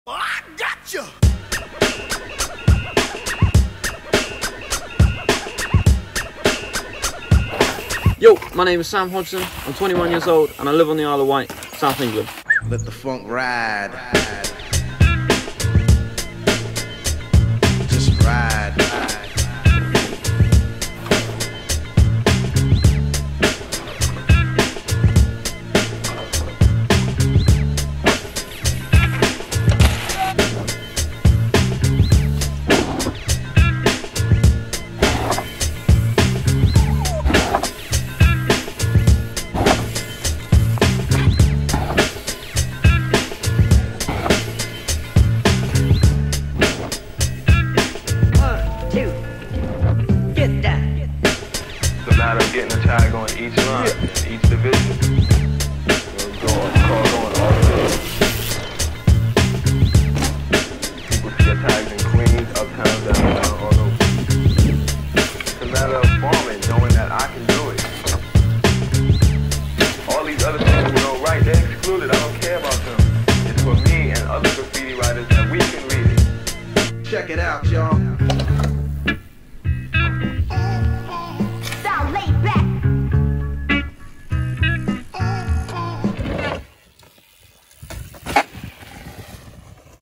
Become A Nine'20 - Sam Hodgson
Here is Sam Hodgson lacing up banger after banger for his Become A Nine submission. Cheers to another ripping contribution!

With almost 40 Become A Nine contenders we're once again mindblown by the crazy talent out there, give us a couple days to check each and every entree out and we'll come with the results soon!